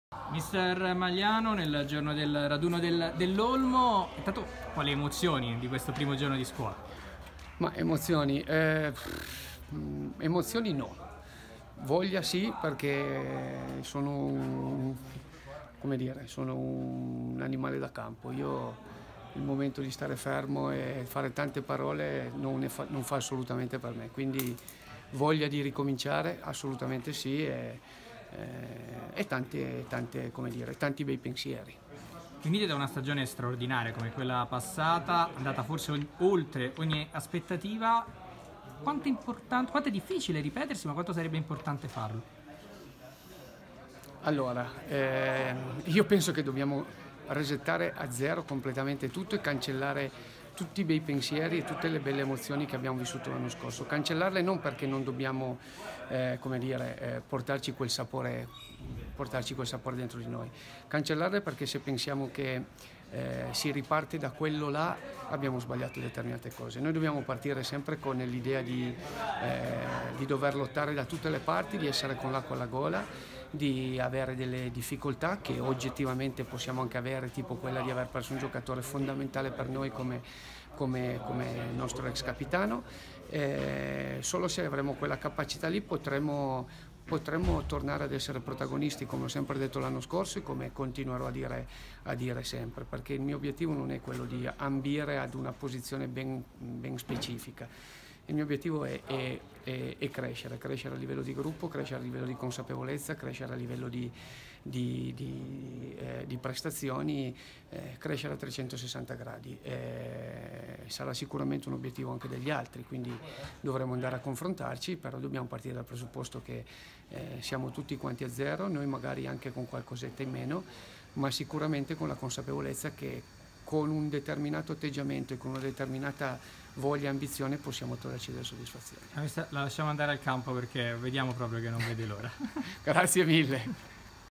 Eccellenza: Olmo, parla Magliano nel giorno del raduno della squadra
L'allenatore della squadra grigiorossa ha parlato ai microfoni di campioni.cn